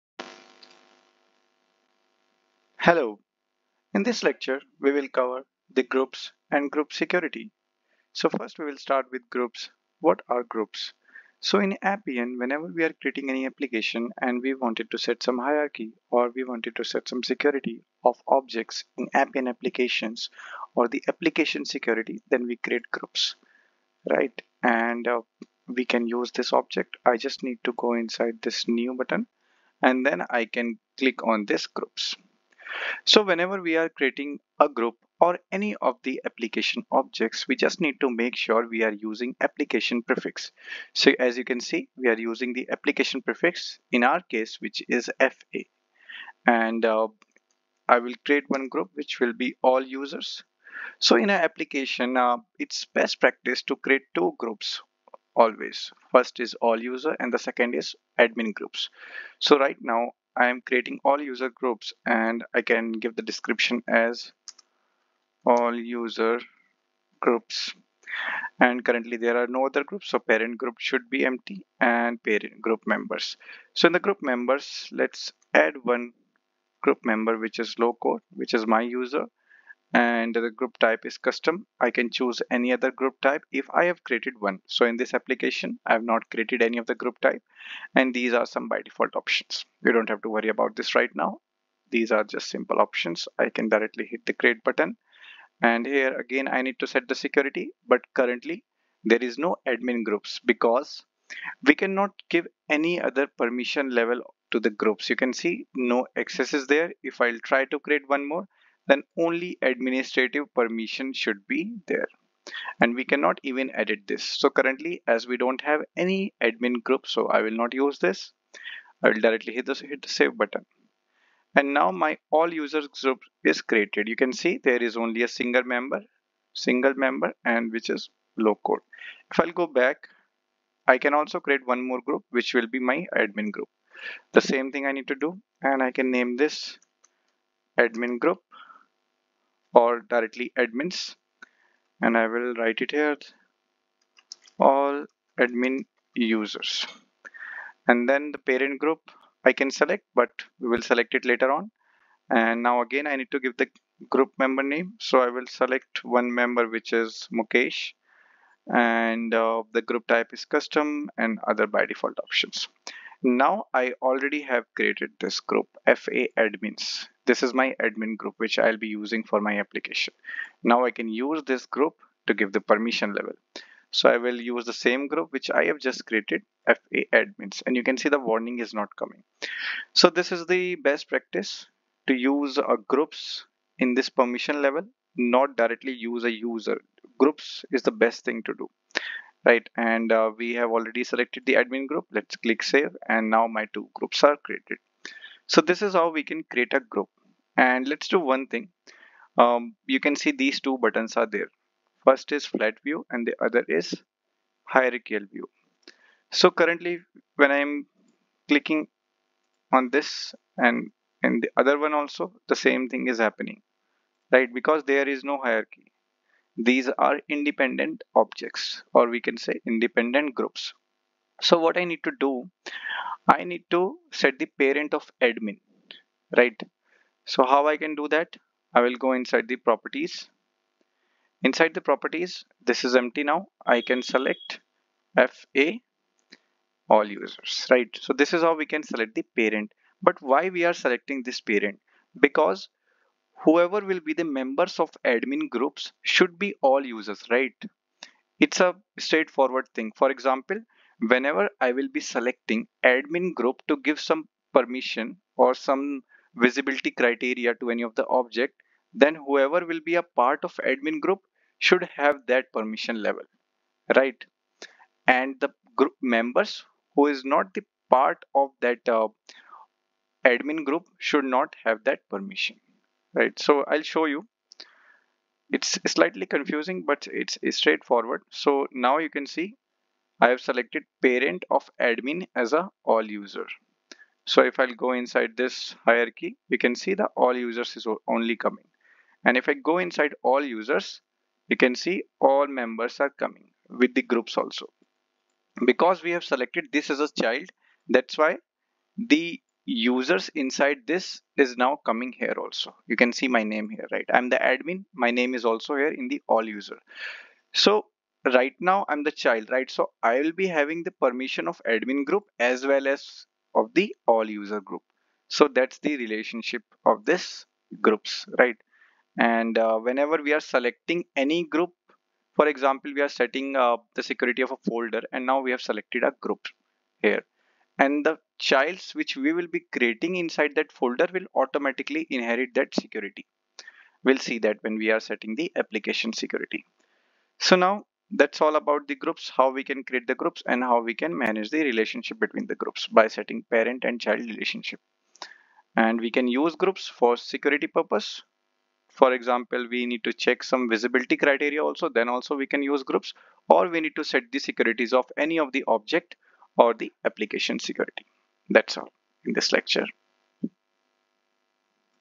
Appian Groups & hierarchy
In this Lecture, we'll learn about Appian Groups & Security Feature